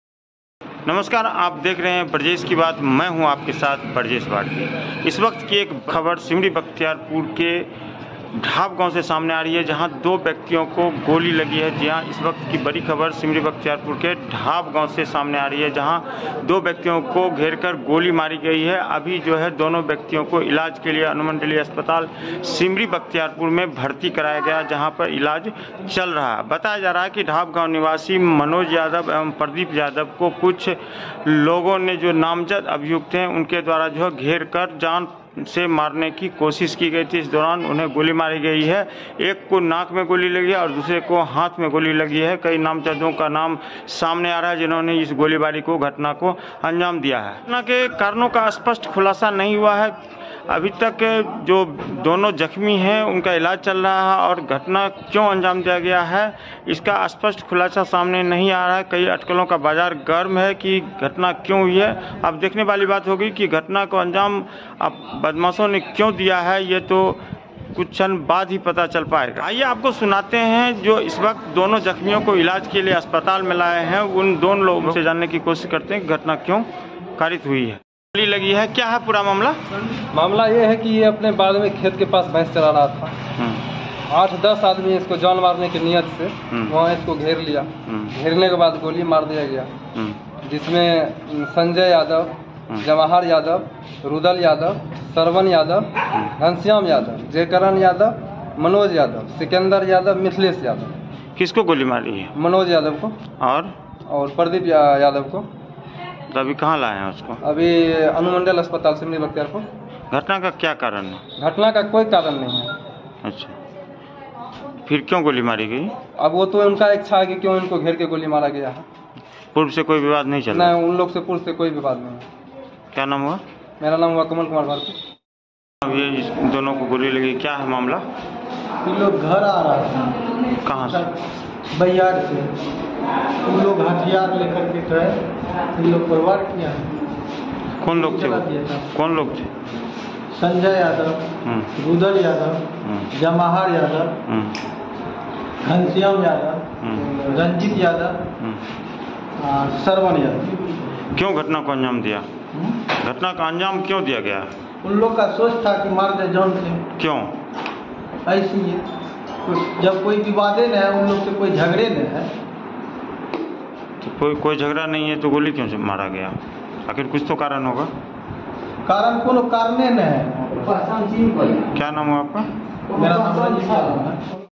ढ़ाव गांव में बदमाशों ने दो किसानों को मारी गोली, अस्पताल में भर्ती, रेफर
@Brajeshkibaat सिमरीबख्तियारपुर खबर को विस्तार से पढने के लिए हमारे वेबसाइट पर लॉगइन करें।